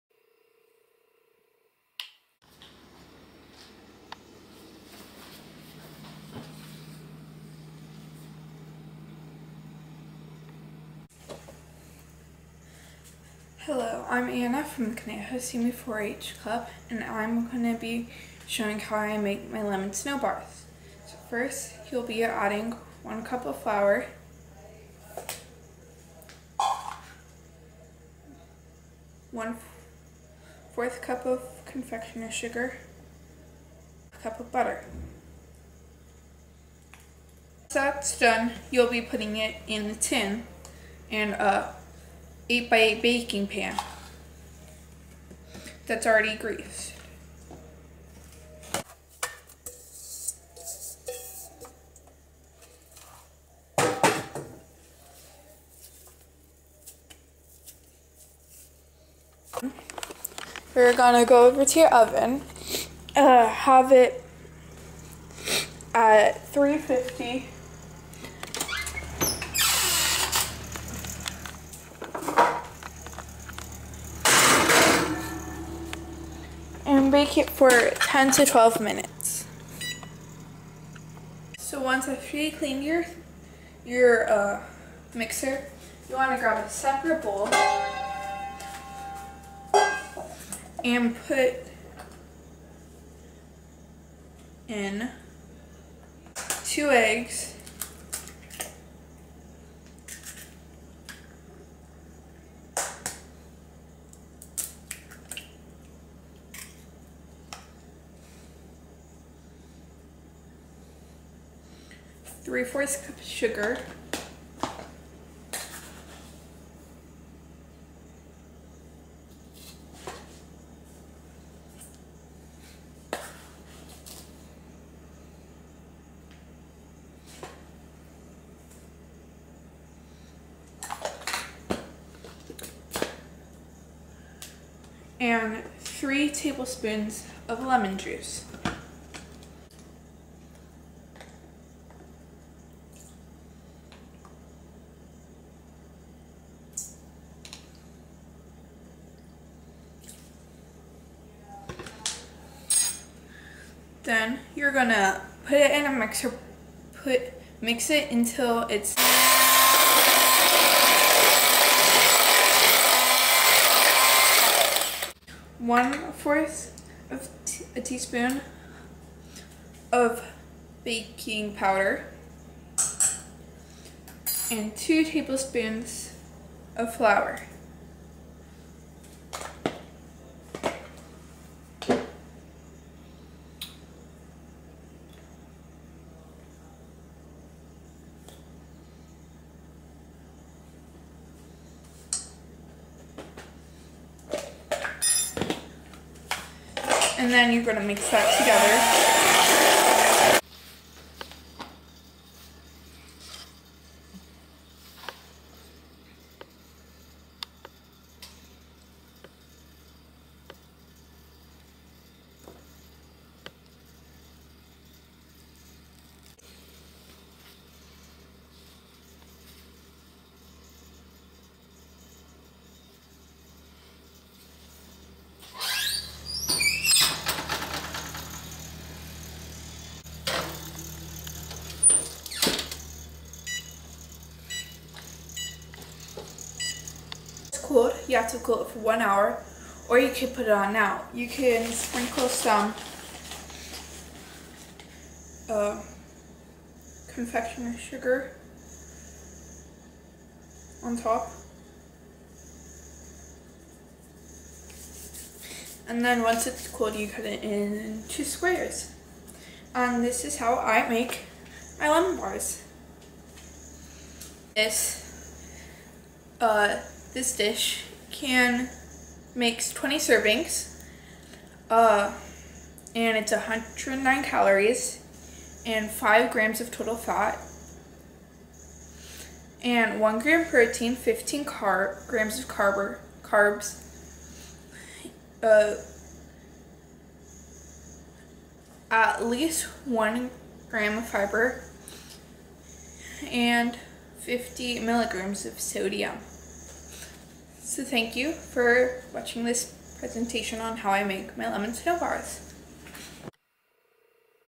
How to Make Snowy Lemon Bars
Anna of Conejo-Simi 4-H demonstrates how to make "snowy lemon bars."